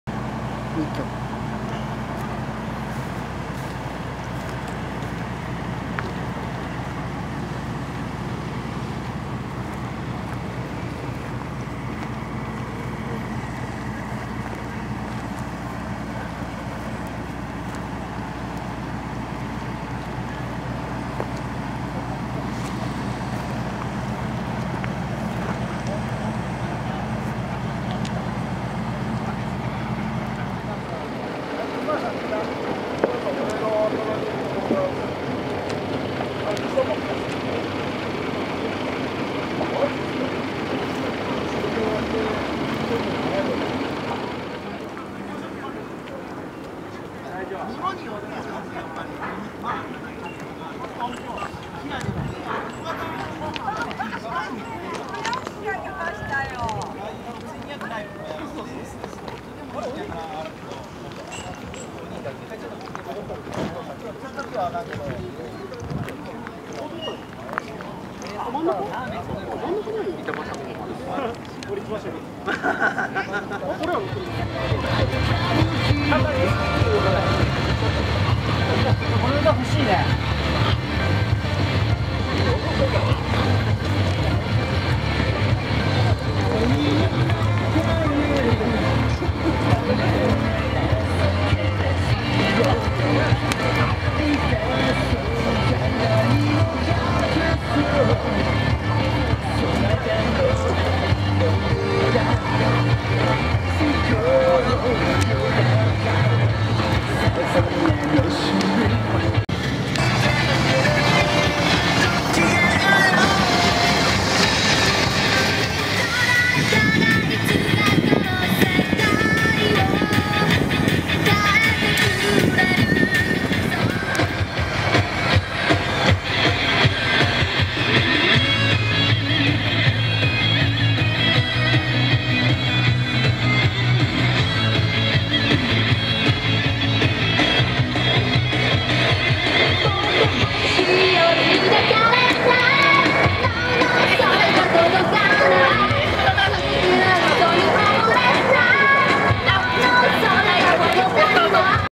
2013年虎鬼船団撮影会 菱星丸 翔龍丸 カスタムオーディオカー
菱星丸さん翔龍丸さんです
手振れピンボケご了承ください
13.12.1　本巣市